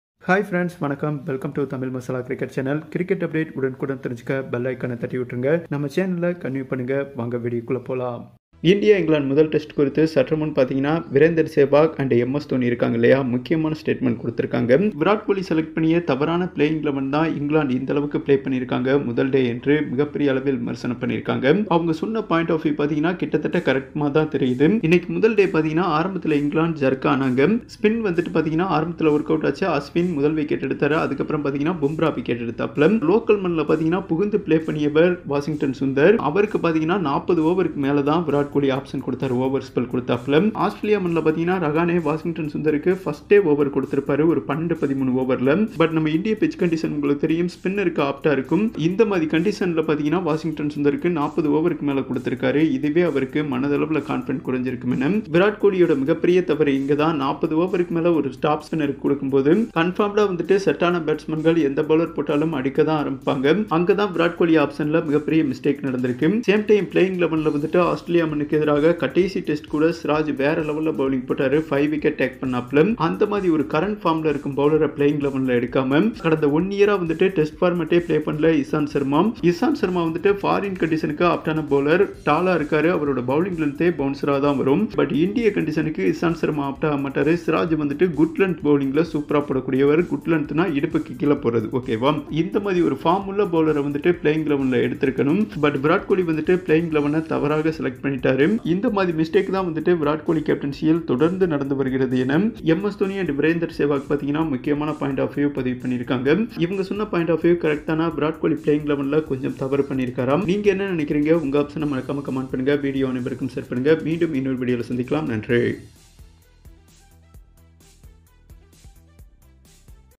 கோலி முடிவுகள் தோல்வியில் முடியும் கொந்தளித்த தோனி & சேவாக் அதிர்ச்சி Report - Ind vs Eng Test Day 1
India vs England, Highlights, 1st Test at Chennai, Day 1, Full Cricket Score: Sibley departs in last over; Visitors 263/3 at Stumps

Ind vs Eng 1st Test Kohli Captiacy Wrong Playing 11 Dhoni _ Sehwag Report